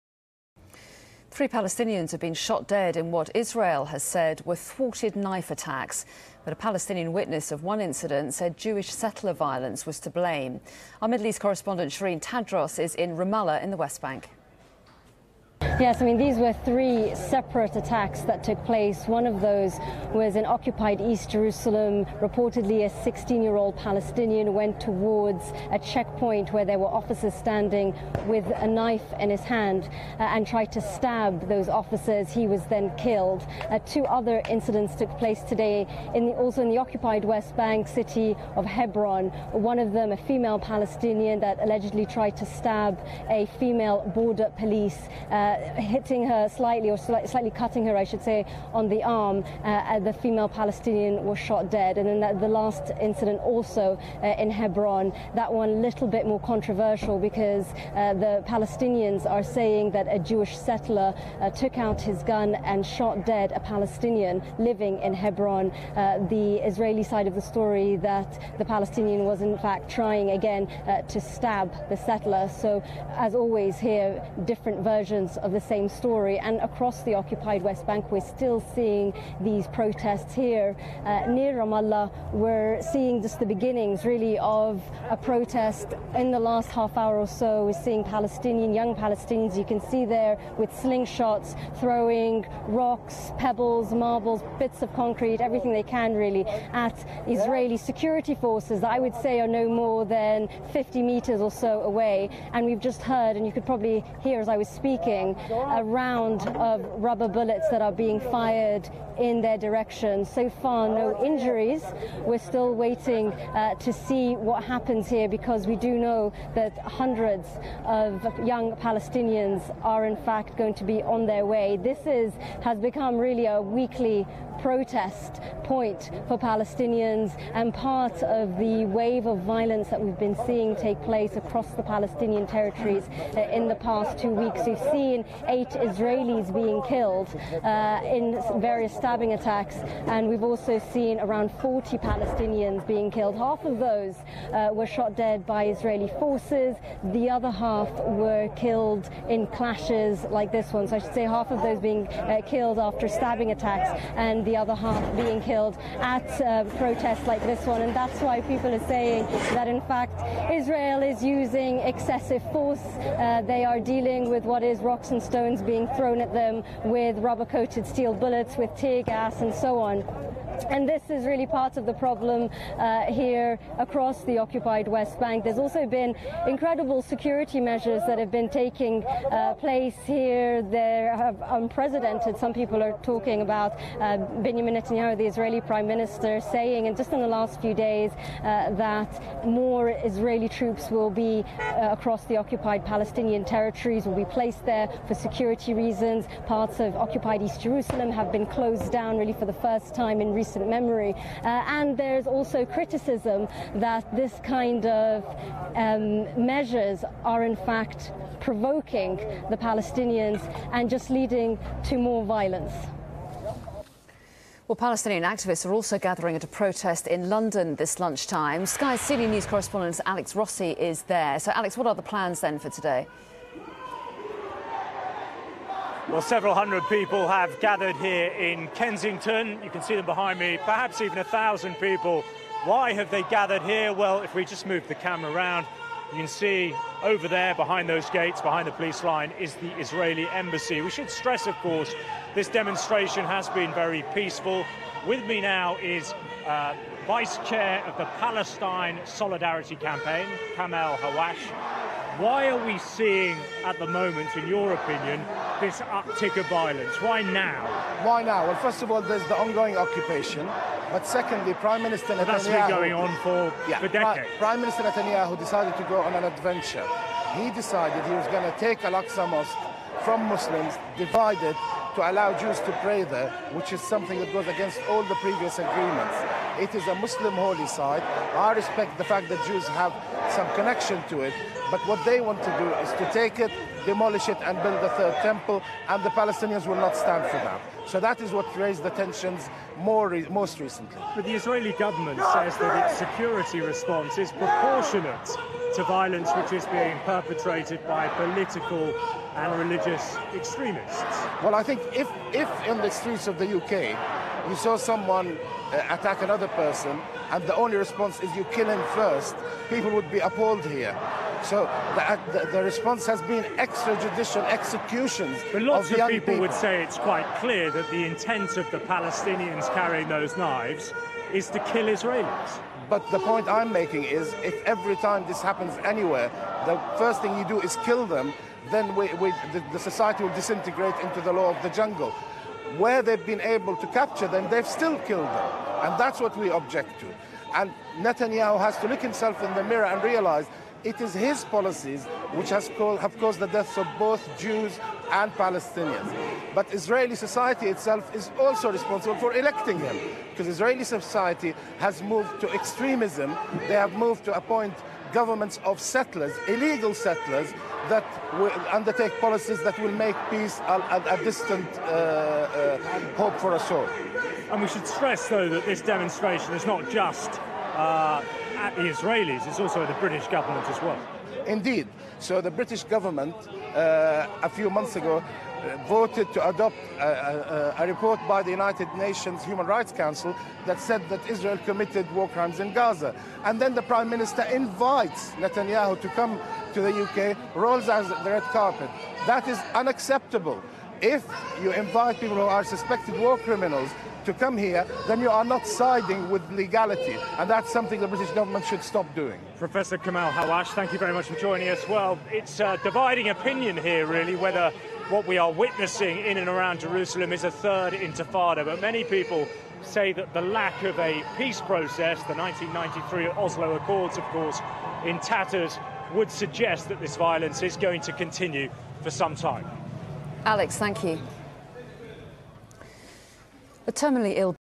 DISGRACEFUL REPORTING FROM SHERINE TADROS OF SKY NEWS
YOU MUST WATCH THIS:
Over the last few weeks I've posted many bad, misleading, biased and unbalanced reports from various news outlets. this is by far the WORST!
Not only do Sky allow their blatantly anti-Israeli reporter Shrine Tadros to report lies, half truths and innuendo but they then allow a member of the PSC the opportunity to tell lies live on national TV.
SHAME ON THEM